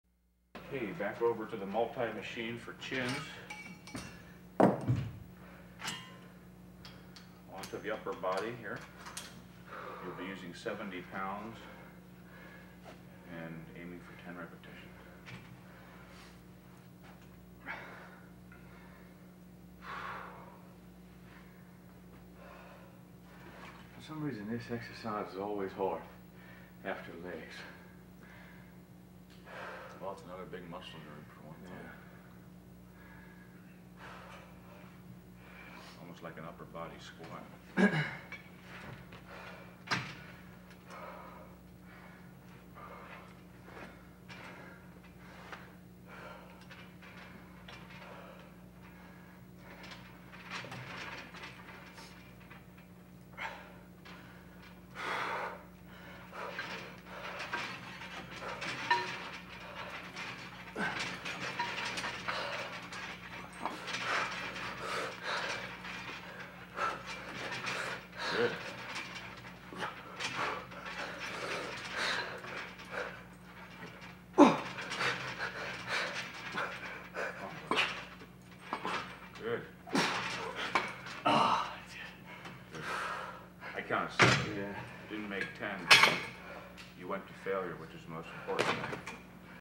Mike Mentzer Trains Boyer Coe Part one
Boyer Coe performing chins @ Nautilus headquarters supervised by Mike Mentzer